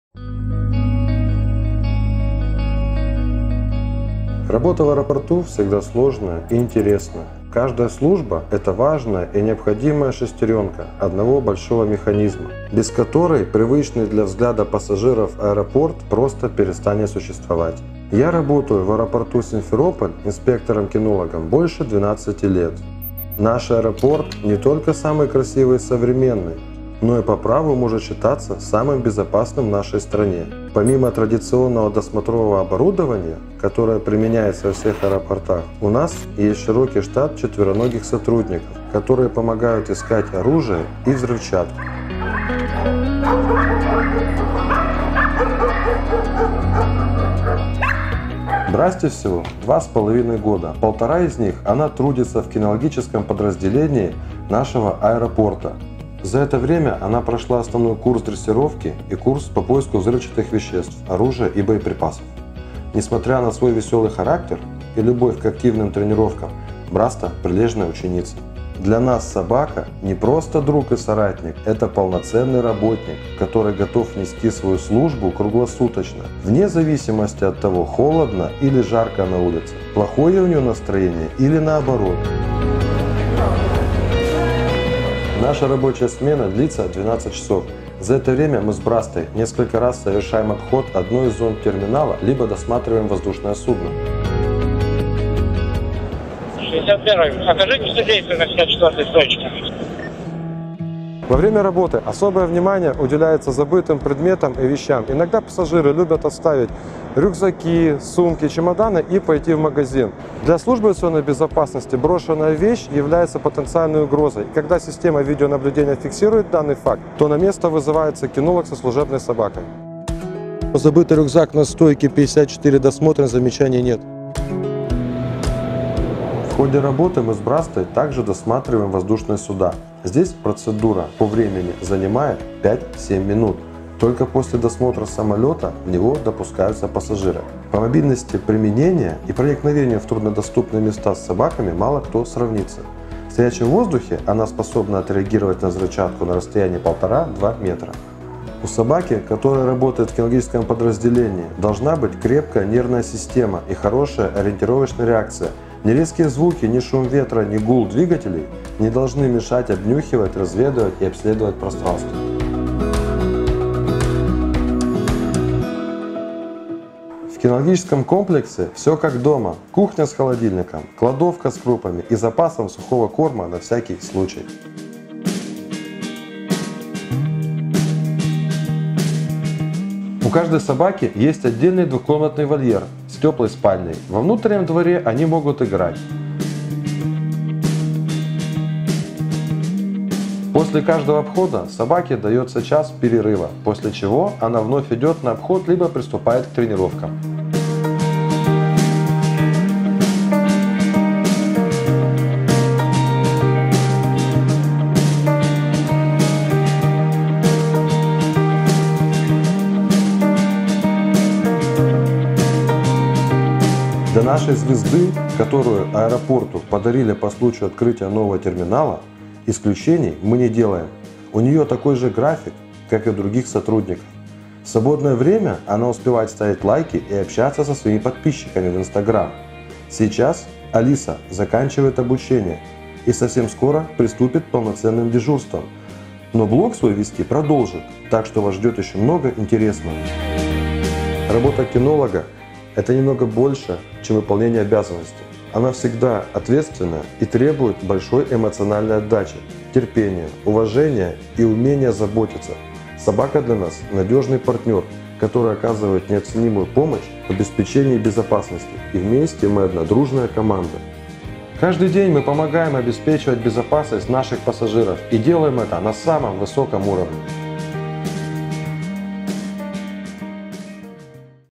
Аэропорт Симферополь. Кинологическая служба.
Если вы всегда мечтали заглянуть в нашу кинологическую службу и посмотреть как проводят свой рабочий день наши четвероногие коллеги, то этот ролик ответит на большинство ваших вопросов!